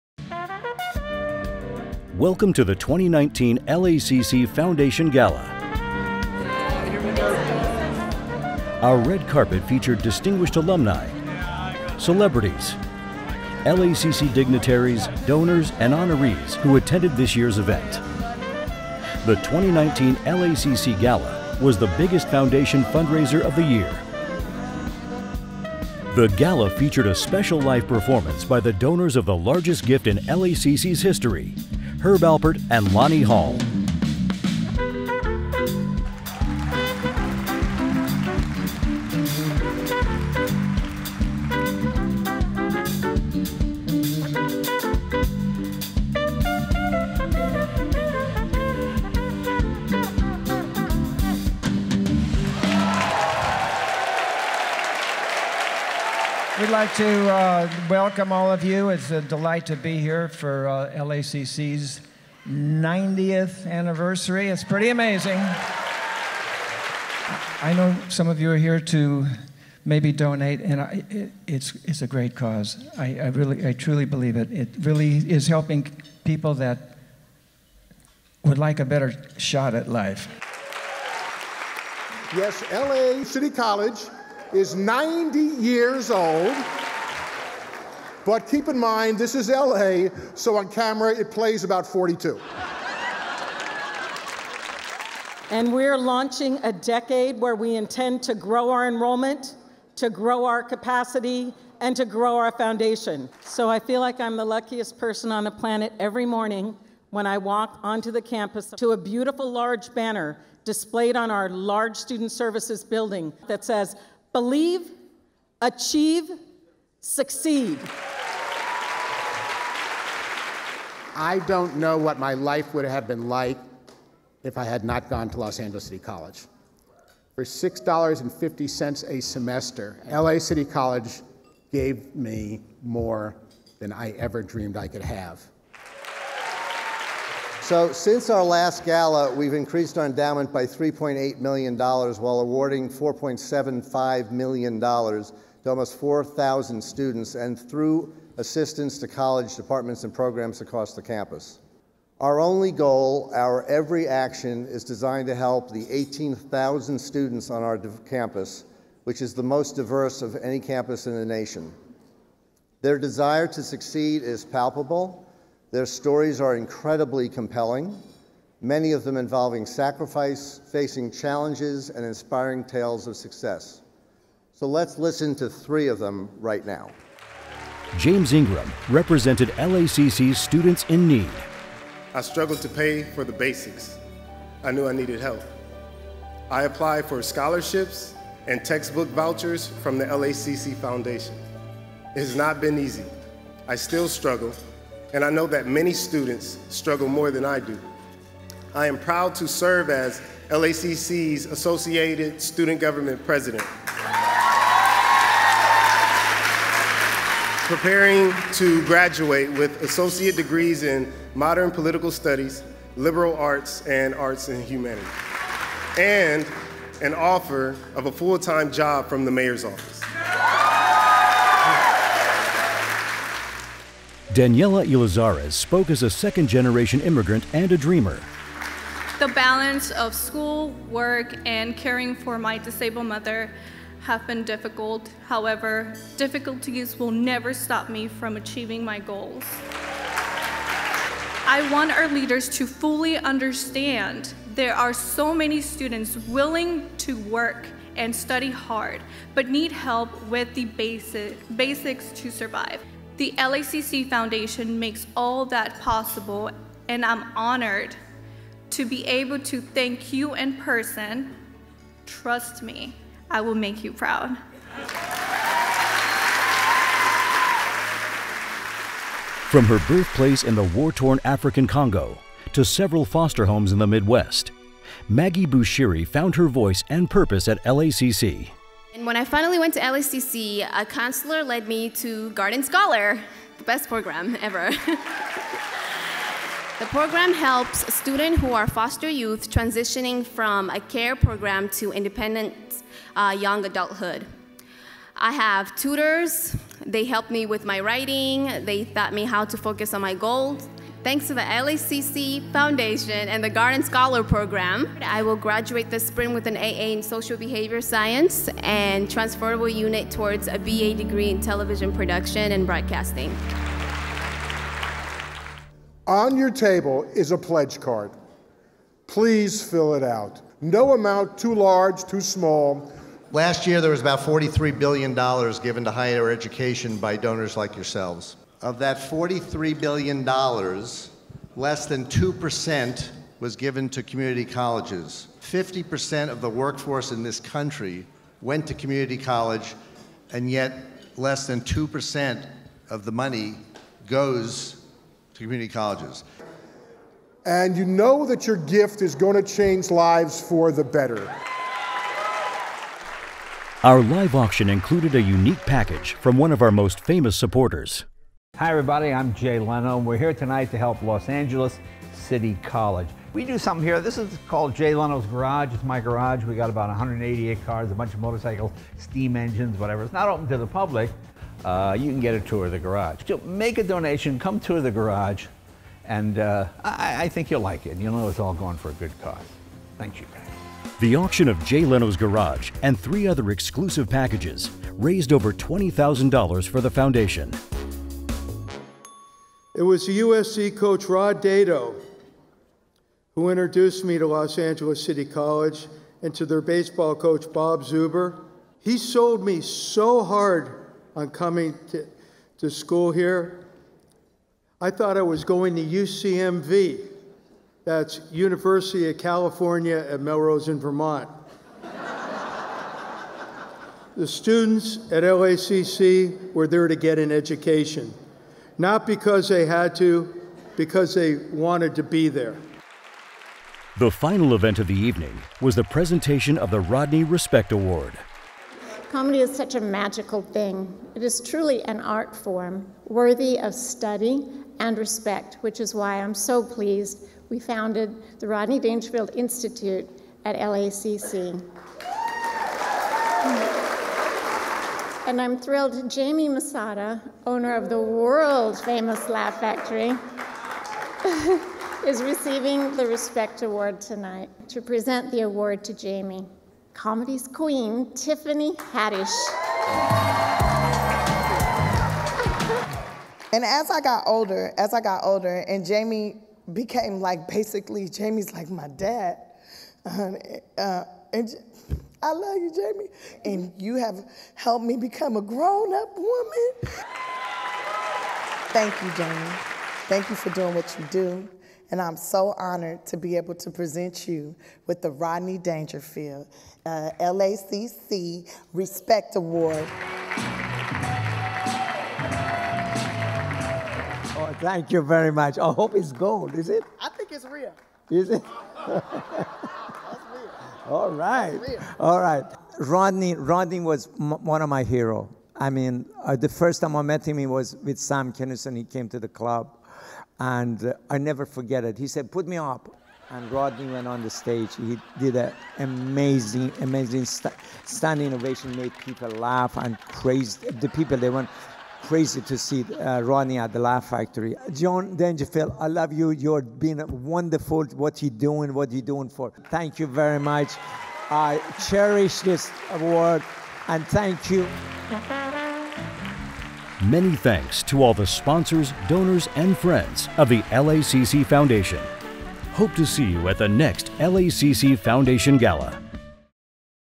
LACC Gala 2019
The Los Angeles City College Gala, celebrating the 90th Anniversary of LACC, took place on Tuesday, March 12 at the Beverly Wilshire Hotel. Tiffany Haddish presented her mentor Laugh Factory owner Jamie Masada with the Rodney Respect Award. CEO and founder of The Gilbert Group Dennis Gilbert and Englander, Knabe & Allen founder and CEO Harvey Englander were honored with the Outstanding Alumni Award. Actor/Comedian Robert Wuhl hosted the event.

This is a yearly event.